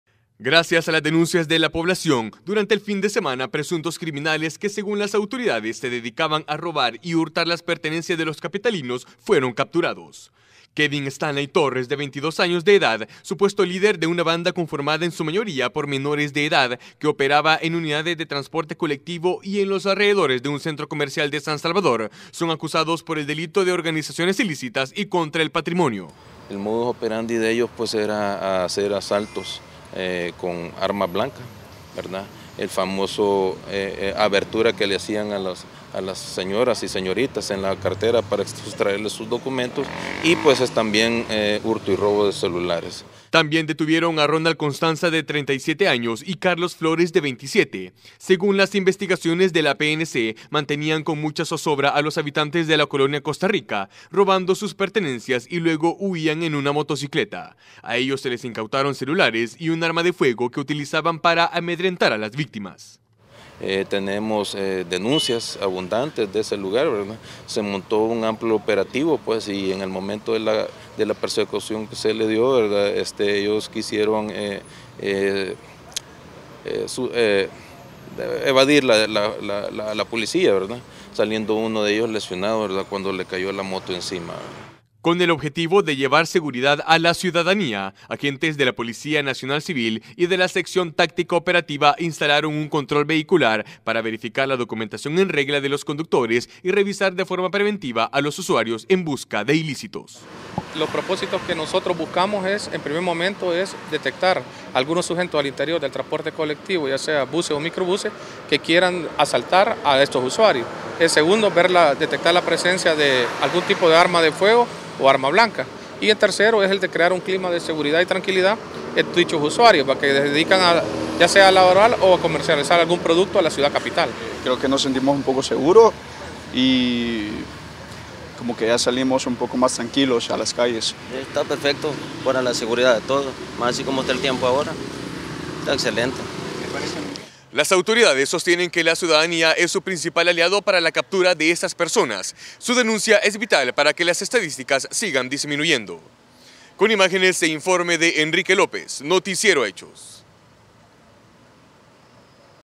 Operativo dejó varias capturas
Una decena de capturas fue el resultado de varios operativos en diversos puntos de la capital este fin de semana.